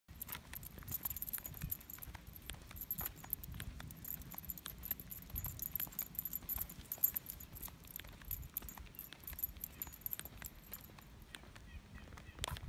Scout struts about her Big Girl Haircut
A 14 week old Welsh Terrier puppy flaunts her new first big girl haircut to the neighborhood